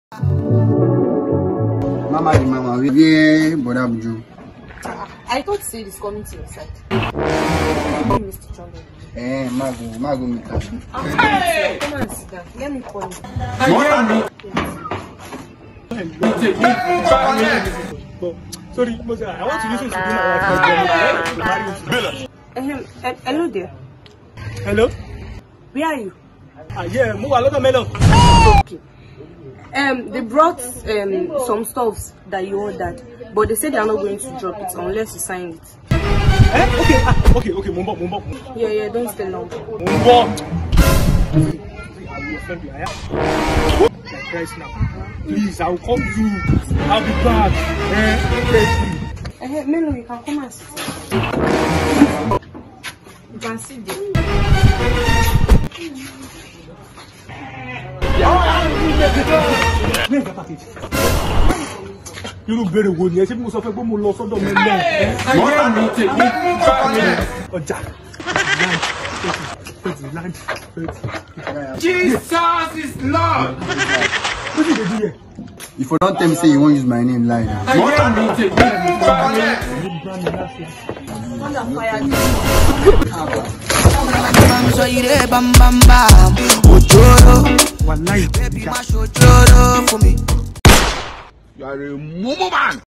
MBBFA - Bag Of Troubles 🎒 (Episode 4)
If you don't want to carry another Bag Of Troubles 🎒 just like MC Broda Bujo 🤡 don't lie to your wife 🧏🏾‍♂️
Remember I warned you oh 🧏🏾‍♂️

See how The umbrella man entered another Wahala 😂